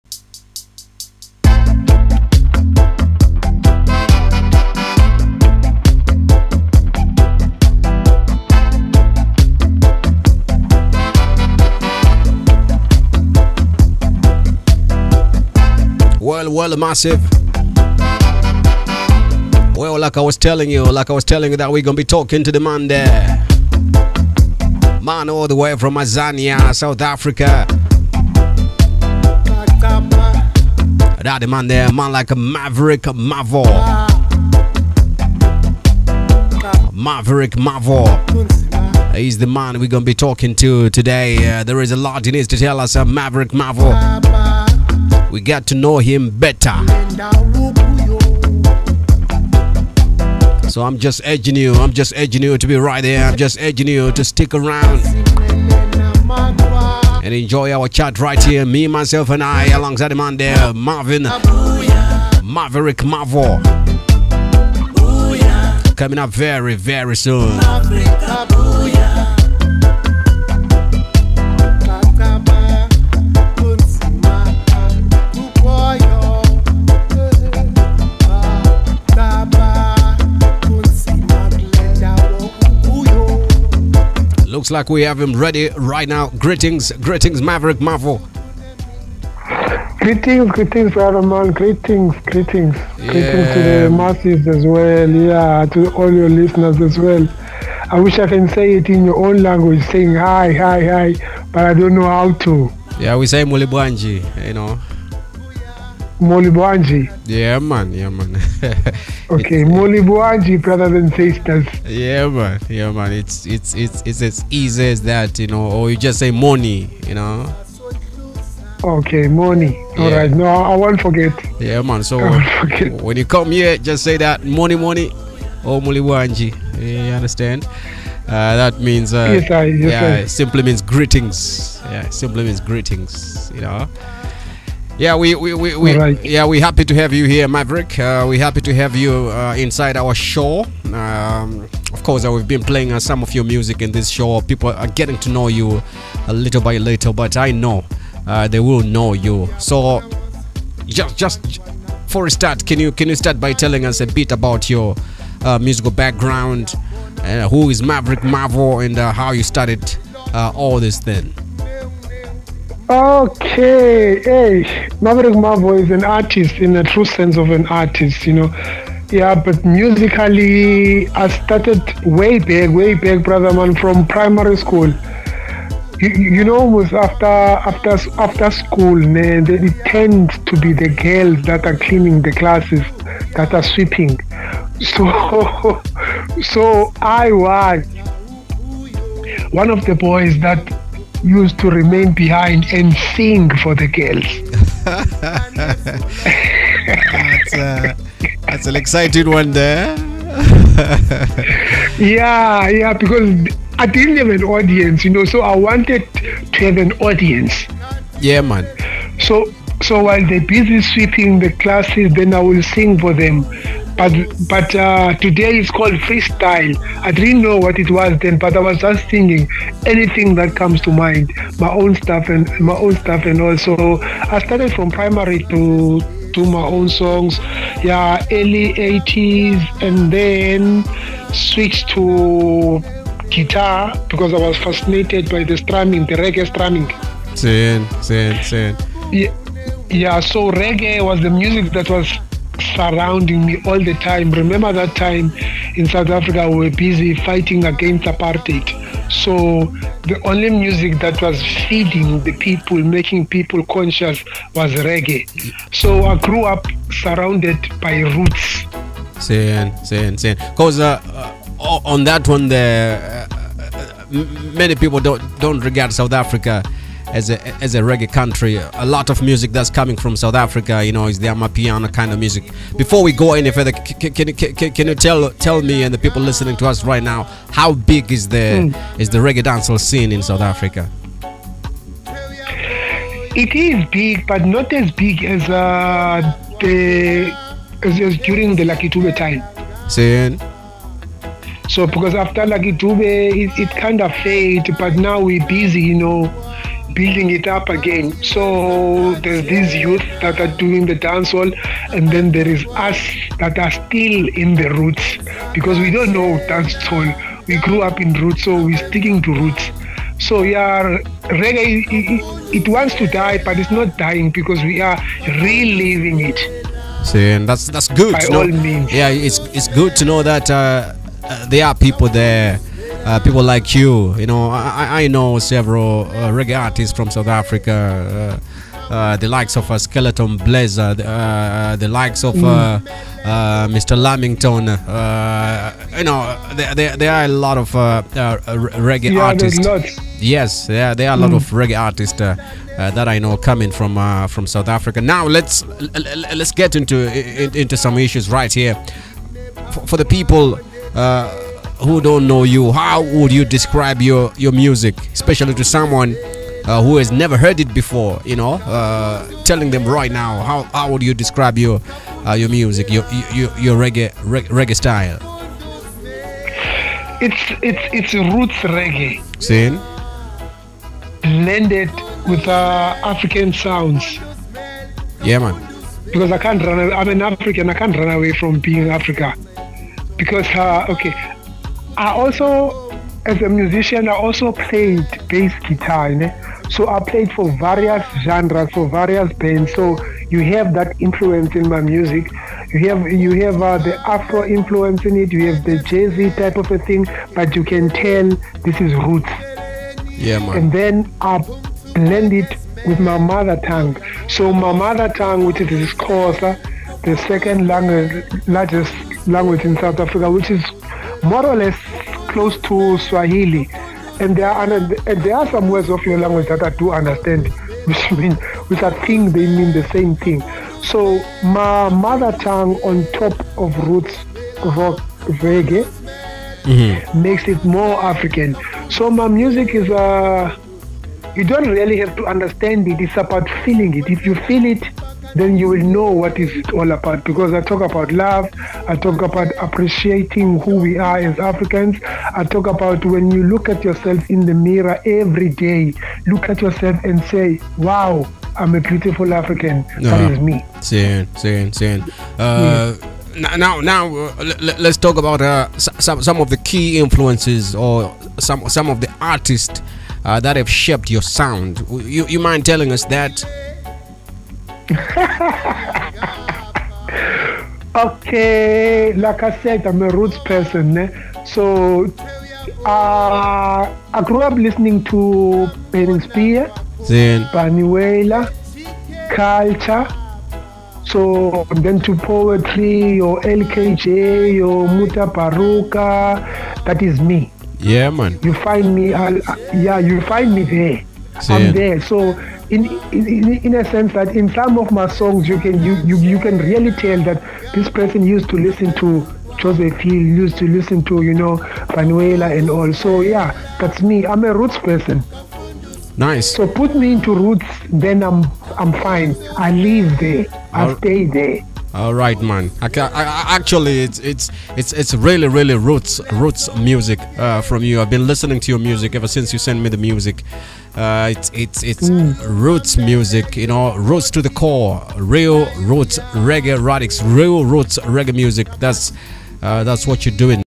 1 of 4 - ROOTS REGGAE TO THE CORE at UMP Festival 2024 (25 - 28 Oct `24), Malawi, with Maveriq Mavo
Dj Spyda from Kasupe Radio (89.2 Mhz) Lilongwe, Malawi digs on how Maveriq Mavo started his music. Where his music is derived from, what is it about, who are his artistc influences, and where to from here?

NEVER FORGET to stream, share, download and tell others to tell others so that they know reggae speaks volumes.